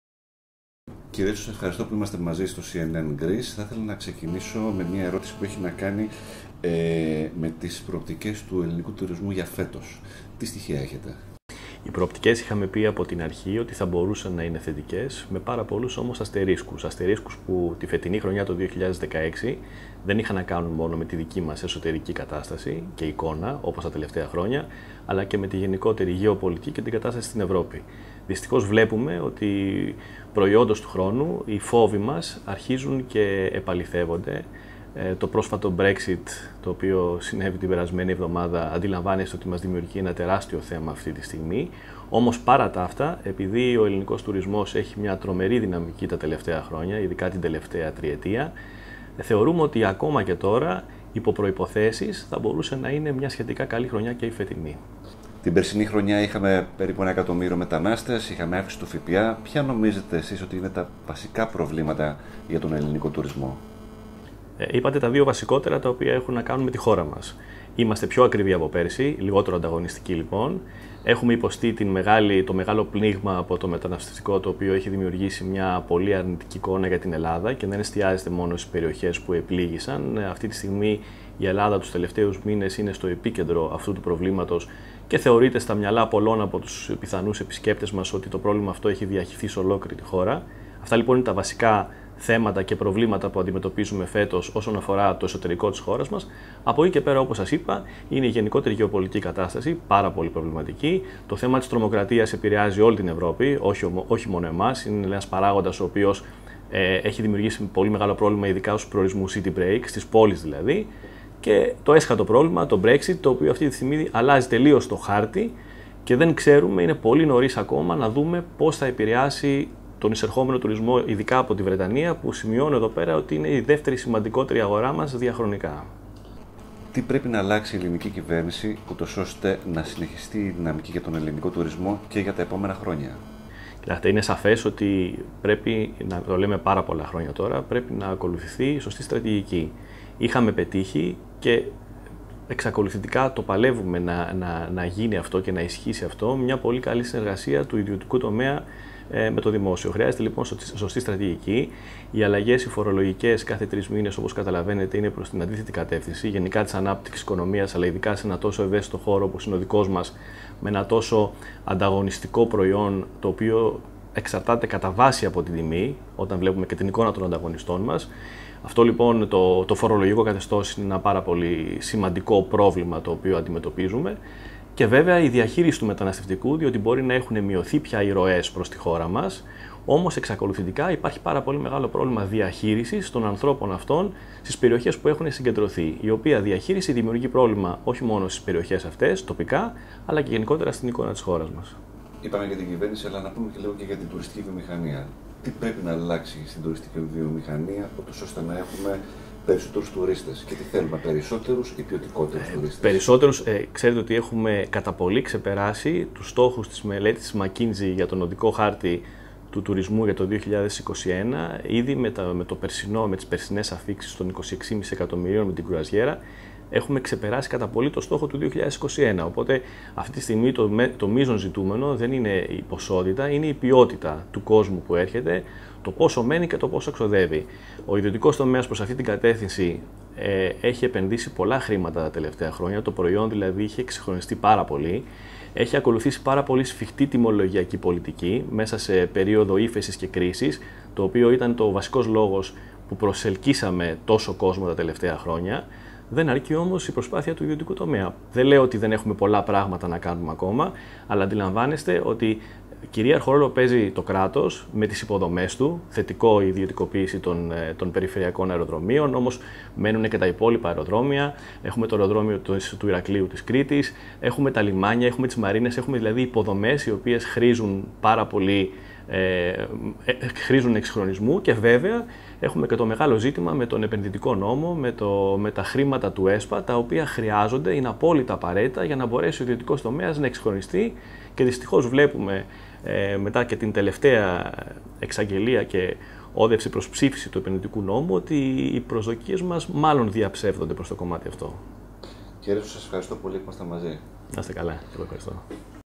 Ιωάννης Ρέτσος Ισχυρός ο τουρισμός αλλά με προϋποθέσεις
O πρόεδρος της Ομοσπονδίας ξενοδόχων μιλά για τις προοπτικές του ελληνικού τουρισμού το 2016